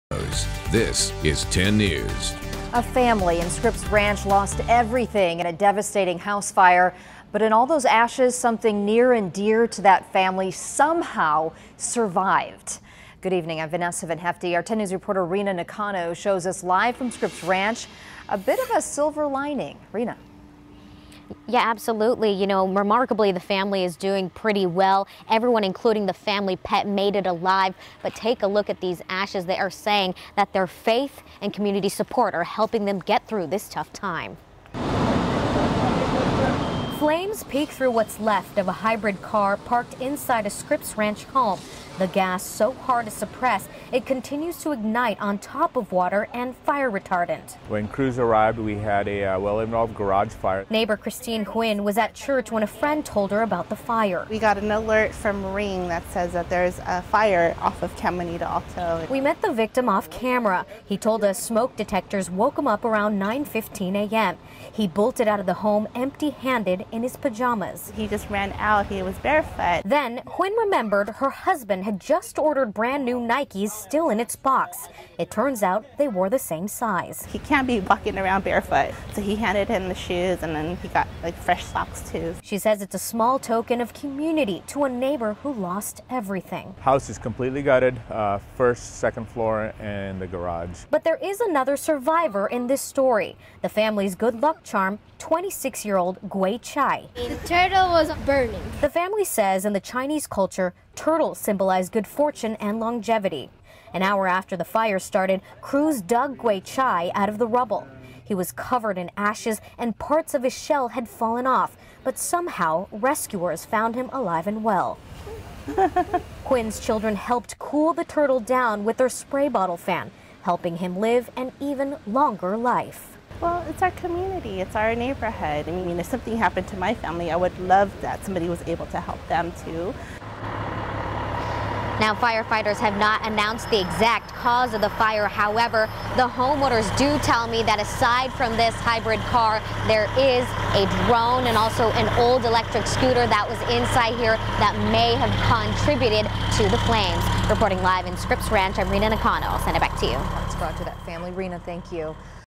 26 year old turtle among survivors of a house fire
26 year old turtle among survivors of a house fire